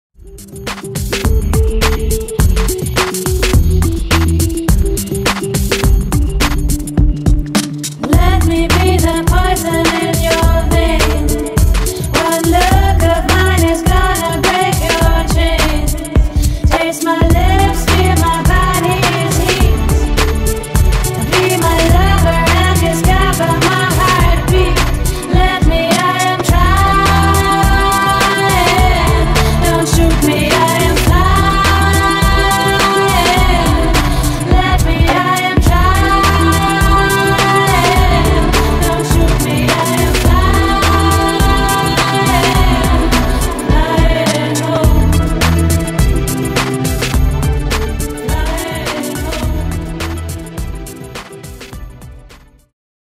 Making of shooting Mim collection Printemps/Eté 2015
Découvrez le making-of du shooting de la collection Printemps/Eté 2015. Une collection estivale aux inspirations ethniques, graphiques et romantiques... Retrouvez toutes ces pièces dans les magasins Mim et sur le E-shop.